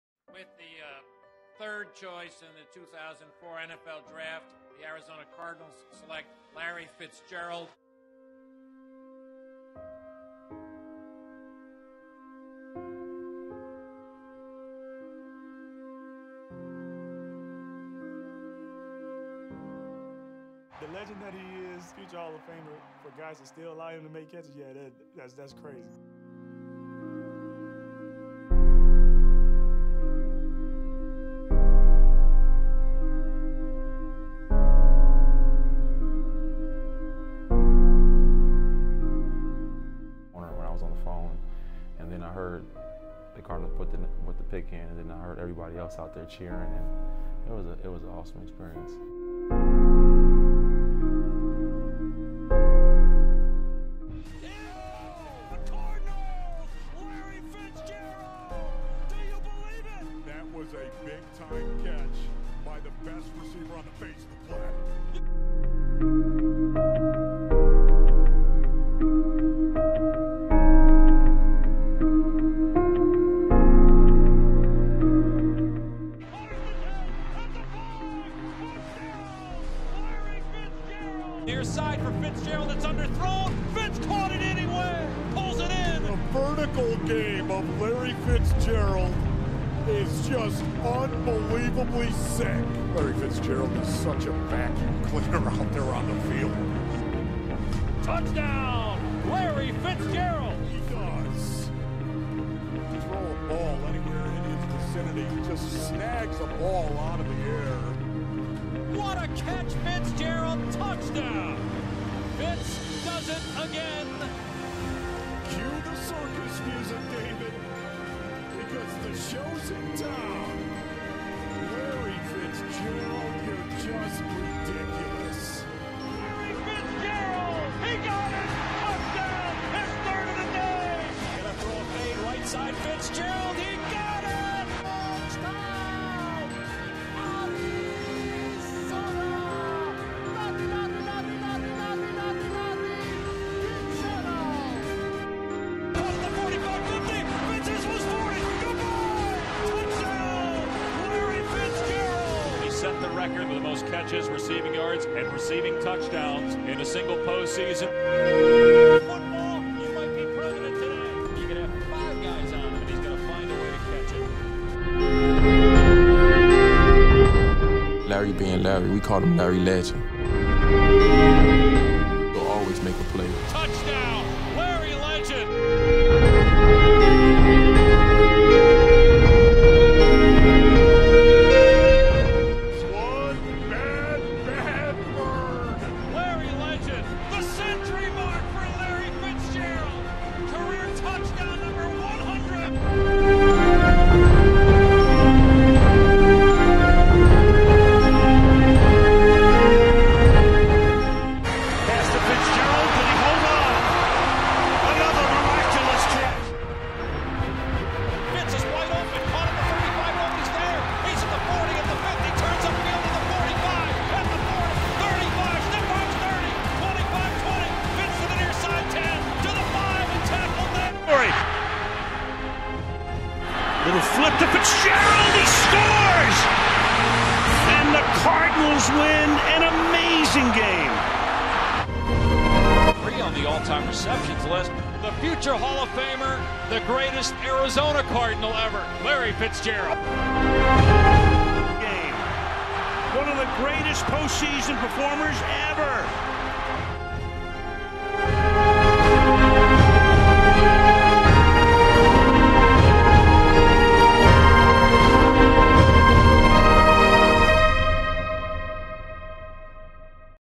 Larry Fitzgerald - "Legend" Trailer
Besides Calvin Johnson, Larry is my favorite receiver ever. He's one of the greatest players ever, and one of the greatest guys ever. I have always looked up to him and respected his humble approach towards football. His work ethic and determination sets him apart from other receivers and shows his love for the game. This video will never do him justice for everything he's accomplished. I pray Larry one day gets the Super Bowl ring that he has always deserved. Thank you Larry for everything you've done for the NFL. - Milkshake